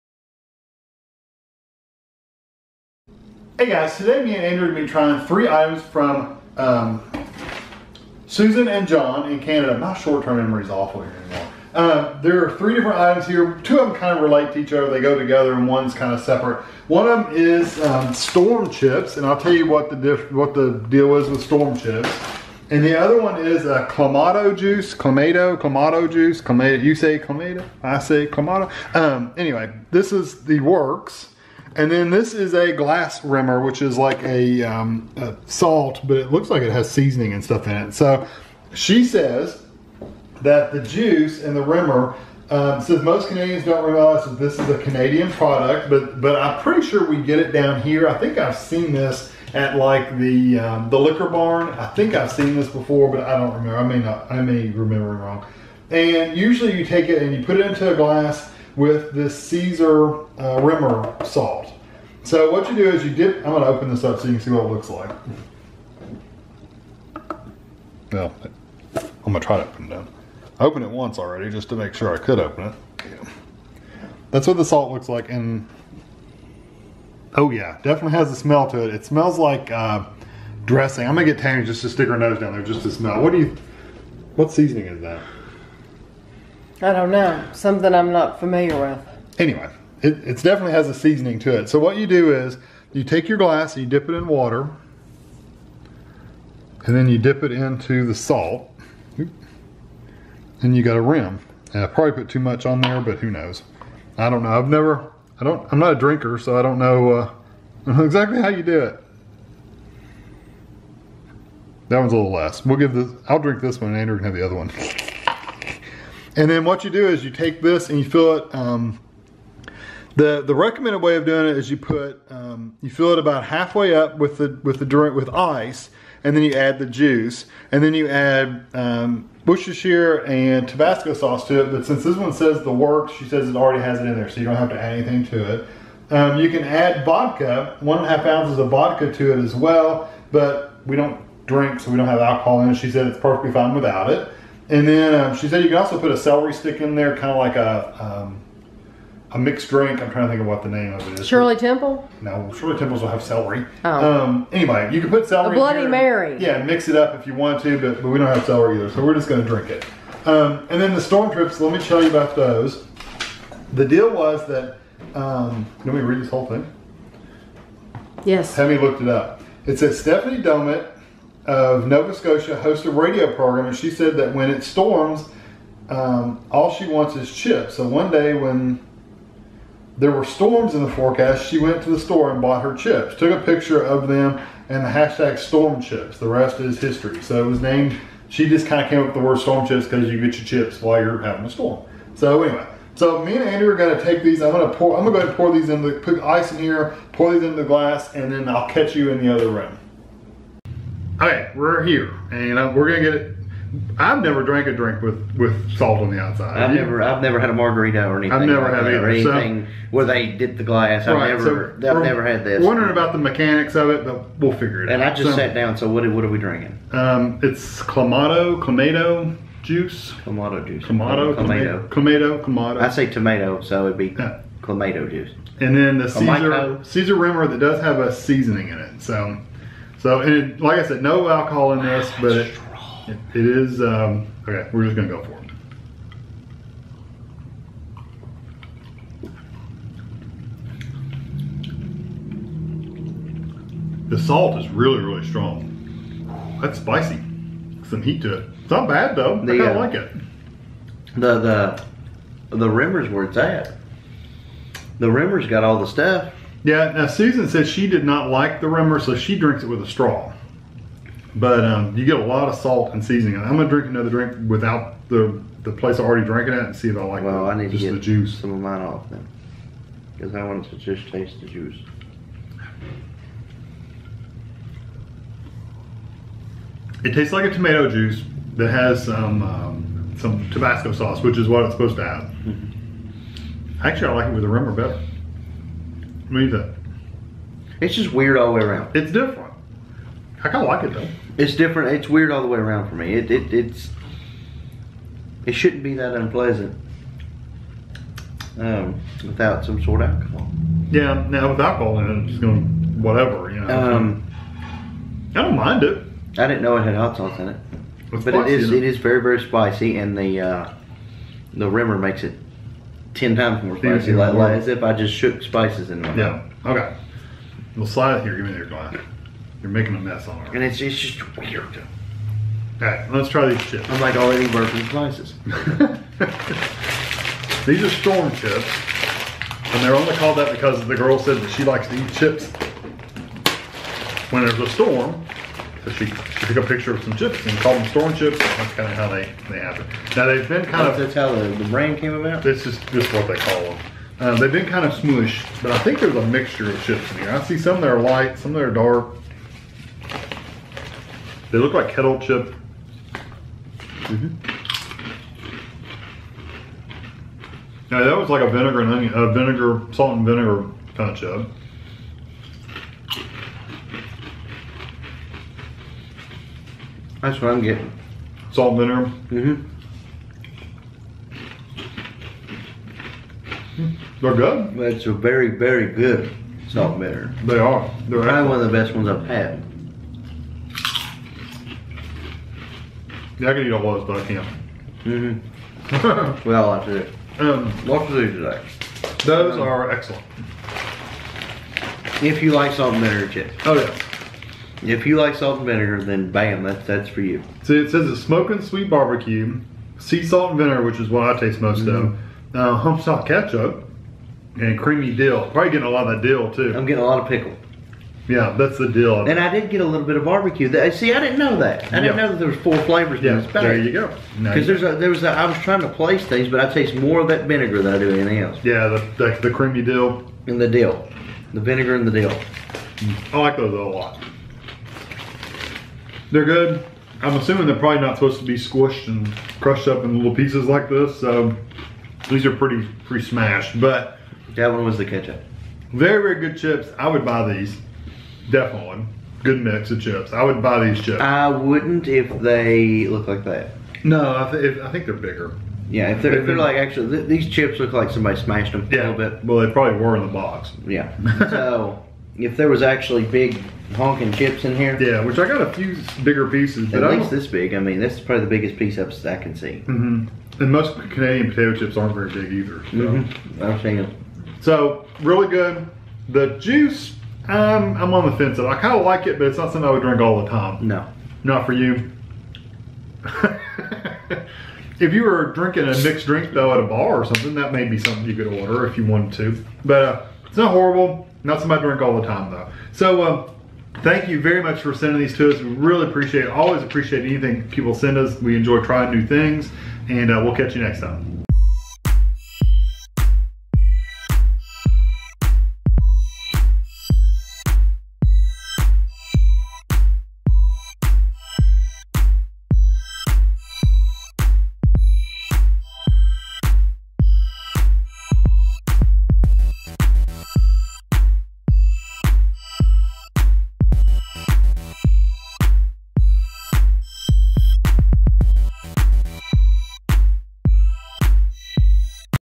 Canada: Covered Bridge Storm Chips, Matt & Steve’s Caesar Rimmer & Mott’s Clamato Review
This is a taste test/review of the Covered Bridge Storm Chips, Matt & Steve’s Caesar Rimmer and Mott’s Clamato. These were sent to us from Susan and John in Canada.
* The Covered Bridge Storm Chips have four flavors of potato chips mixed in one bag. Flavors include: Smokin Sweet BBQ, Sea Salt & Vinegar, Homestyle Ketchup and Creamy Dill. 29 chips (50g) = 250 calories
* The Matt & Steve’s Caesar Rimmer includes spices, garlic and salt.
* The Mott’s Clamato is a tomato clam cocktail. 250 ml = 60 calories